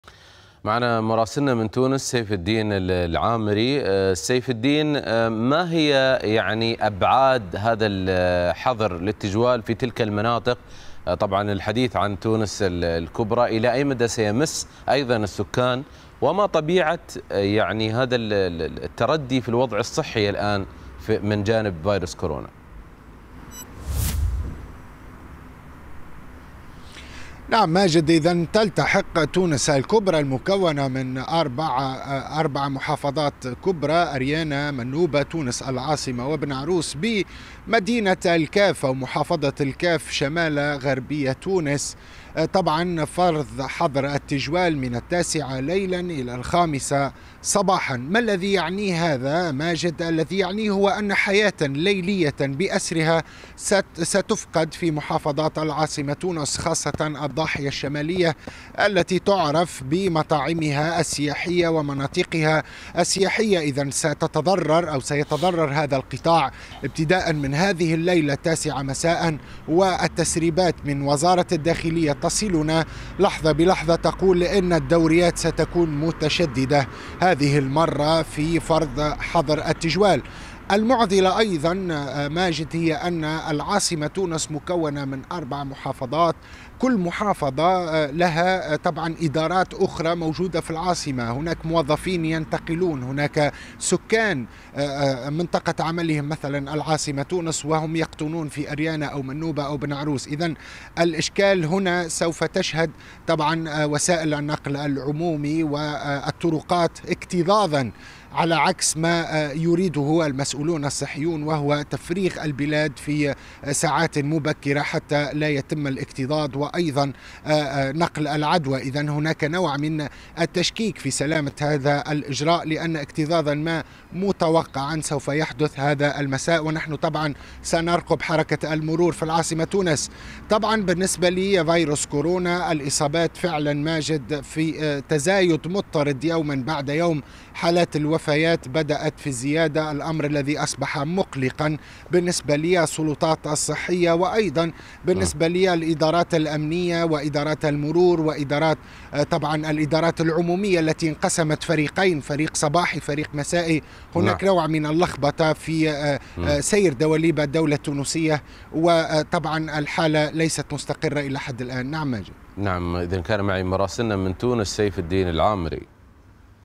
قرار بفرض حظر التجول في محافظة تونس الكبرى بسبب تفشي كورونا
من المقرر فرض حظر للتجول الليلي في محافظة تونس الكبرى، بدءاً من اليوم سعيا للحد من انتشار وباء كورونا. يأتي ذلك بعدما دعا حكام أربع ولايات، تُشكل محافظة تونس الكبرى، إلى اتخاذ إجراءات لوقف الارتفاع في حالات الإصابة بكورونا... المزيد من التفاصيل يتحدث مراسلنا من تونس سيف الدين العامري، مع ماجد الفارسي.